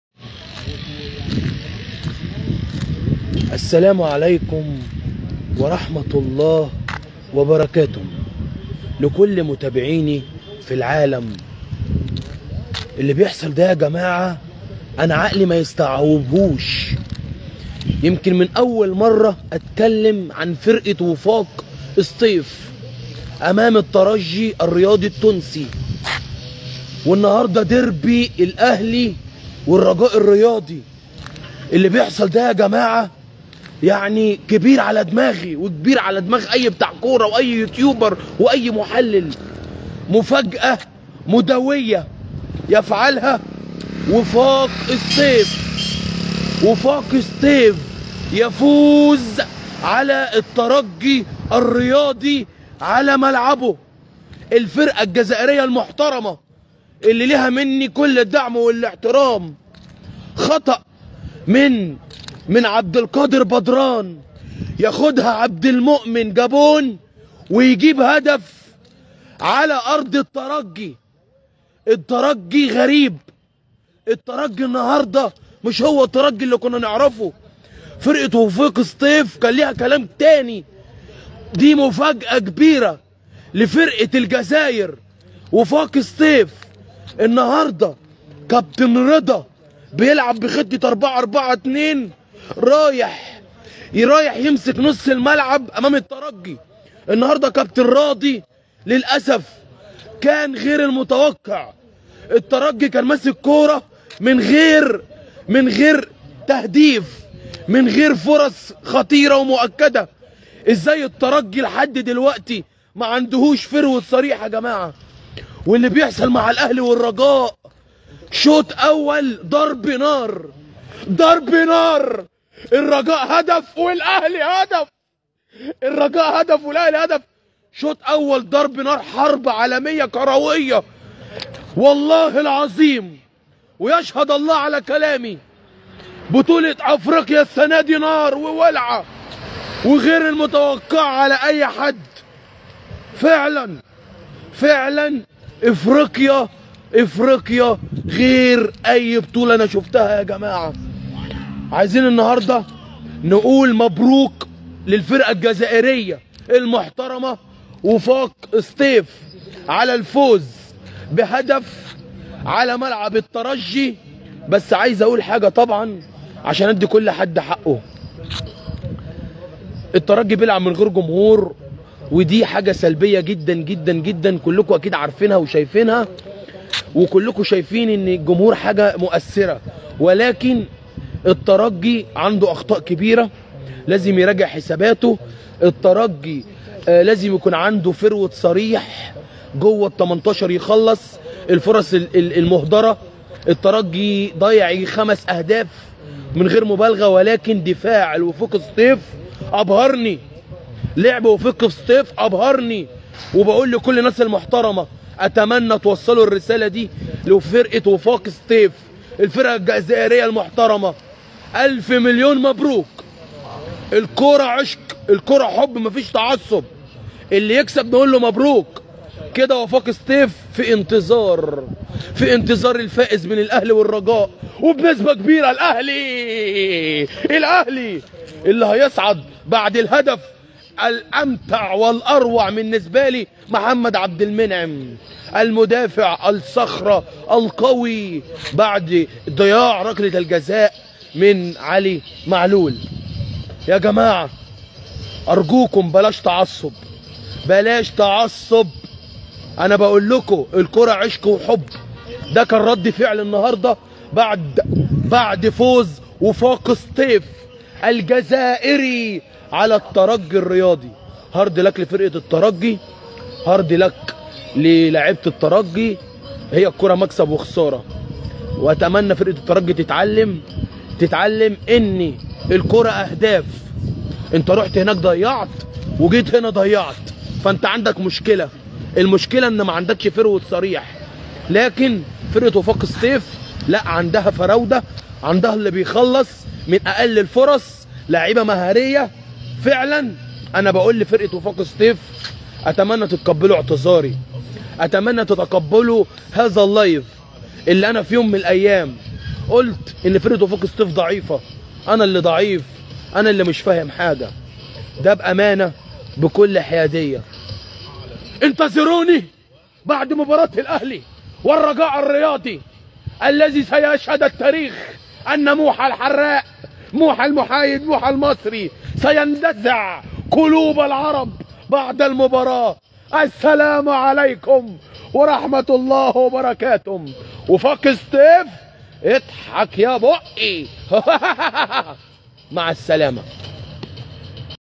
حصري وفاق سطيف يفوز على الترجي الرياضي و يصعد نصف النهائي رد فعل موحا المصري الحراق 2022
est vs ess
caf can afrique coupe d'afrique
taraji mkachkha
egypt algerie tunisie marroc
football comedy